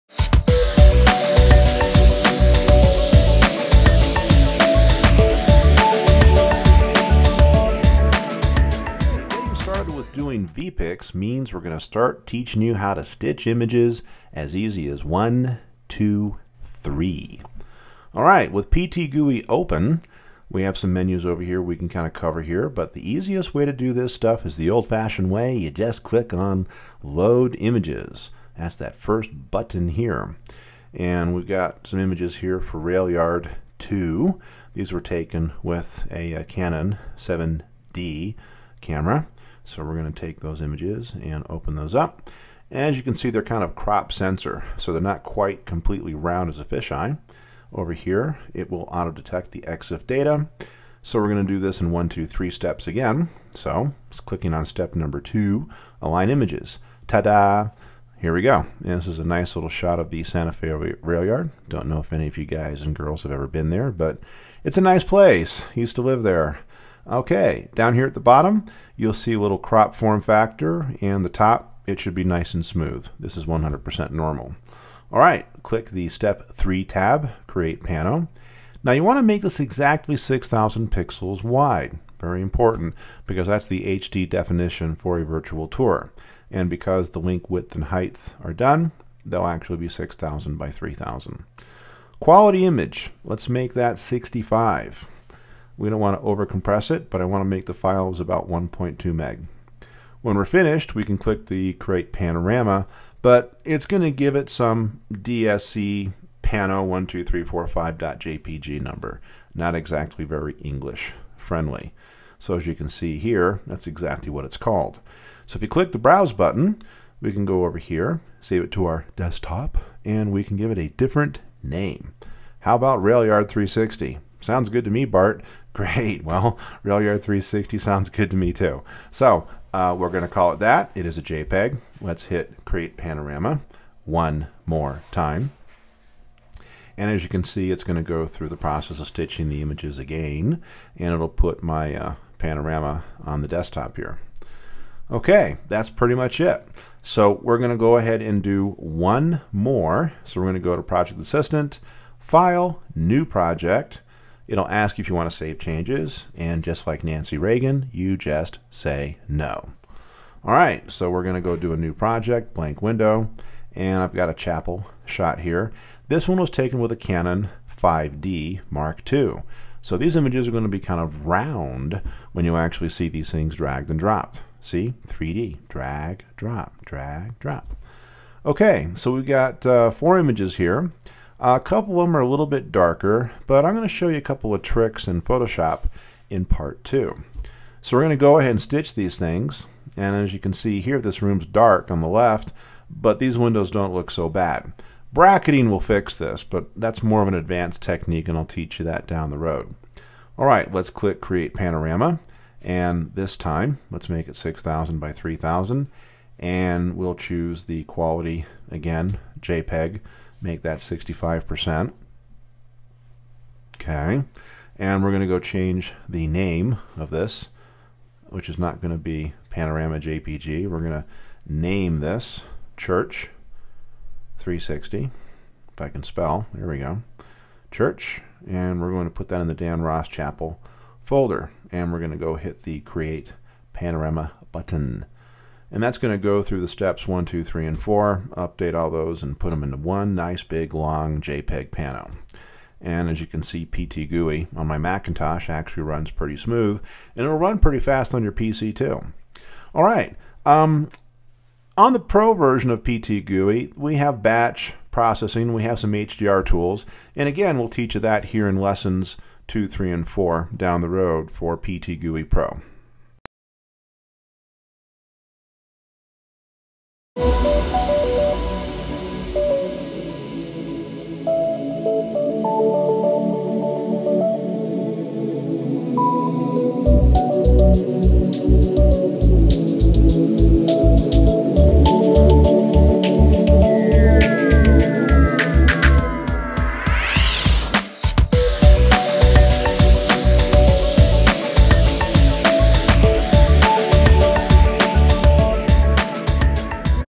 PT GUI Basic Panorama Stitching
Learn how easy it is to stitch full frame or crop factor images taken with a Sigma 8mm f/ 3.5 lens. Bart from VPiX is back and demonstrates both techniques. For advance HDR training with bracketed images and PhotoMatix Pro, be sure to take their HDR Advanced class. For more information on virtual tour training in HDR, visit VPiX360.com for details.